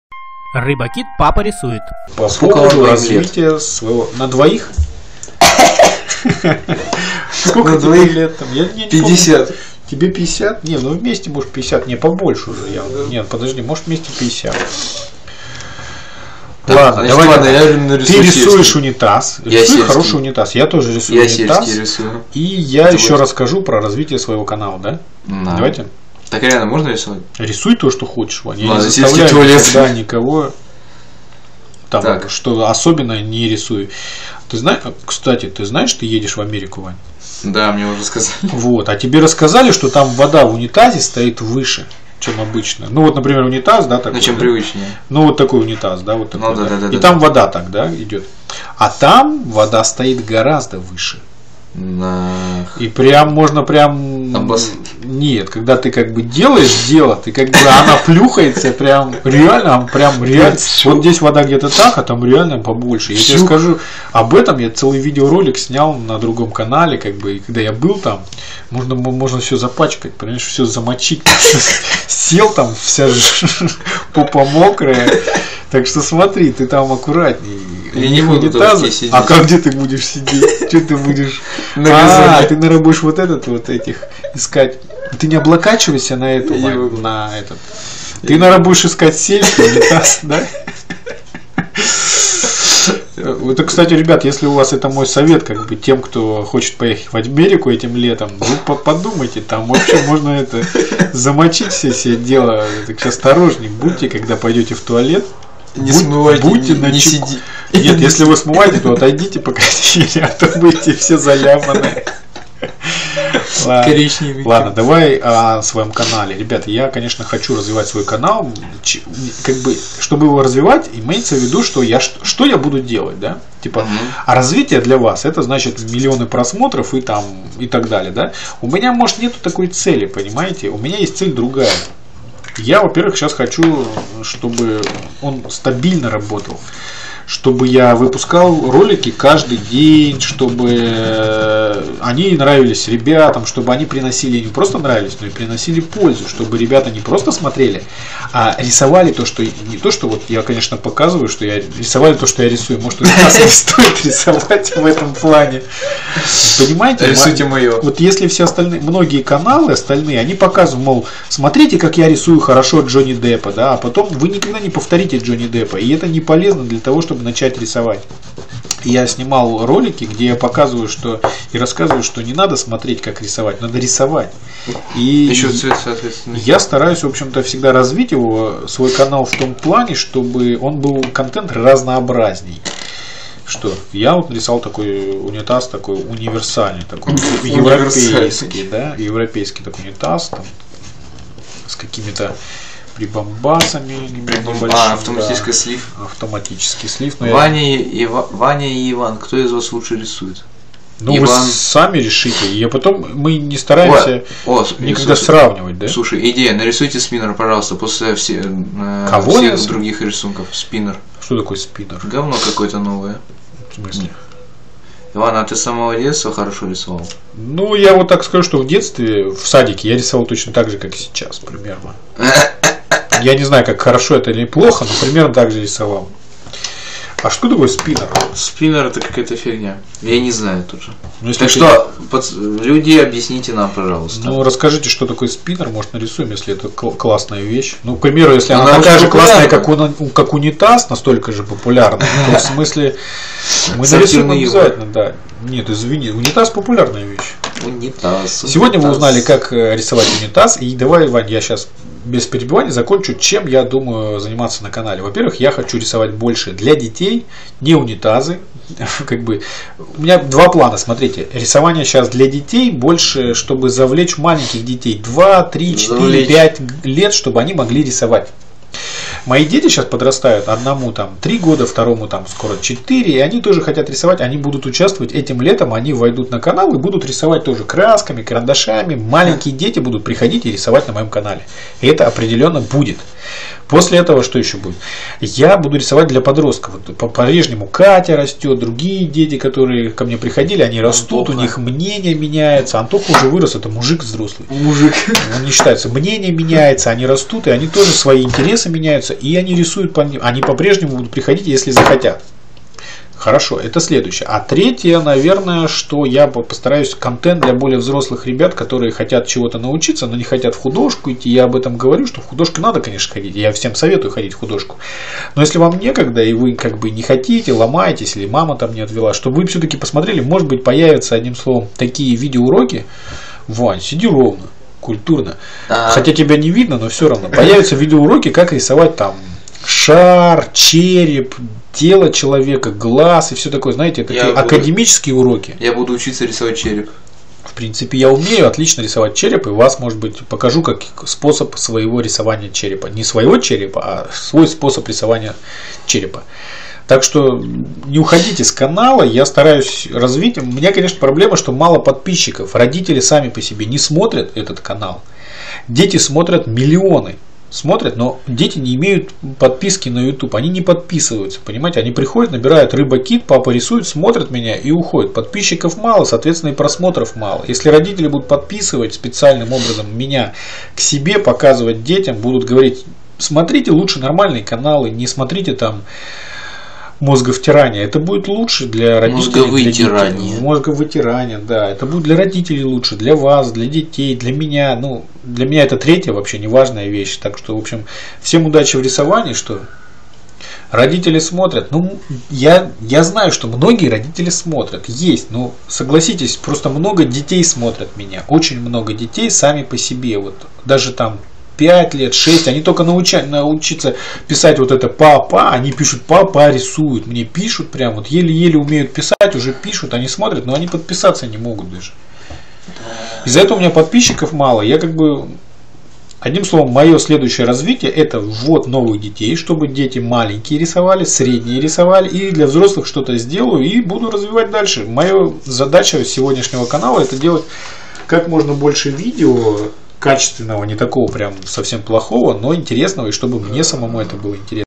Планы развития канала РыбаКит и УНИТАЗ
РыбаКит BOX - УРОКИ РИСОВАНИЯ ДЛЯ ДЕТЕЙ + МАТЕРИАЛЫ (набор для творчества)
Важное выступление меня, как создателя канала РыбаКит. В ролике я высказал, что я хочу сделать на канале, как его хочу развивать дальше.
Это кусок трансляции с канала РыбаКит папа рисует.